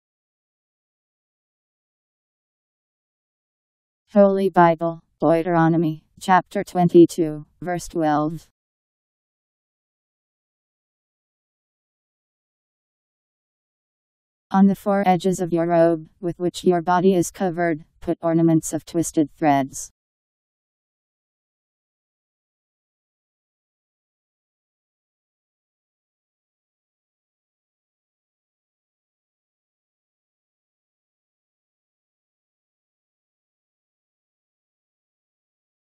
Holy Bible Deuteronomy Chapter 22 Verse 12
Video shows Holy Bible Deuteronomy Chapter 22 Verse 12: On the four edges of your robe, with which your body is covered, put ornaments of twisted threads. Powered by MaryTTS. Text taken from Bible in Basic Edition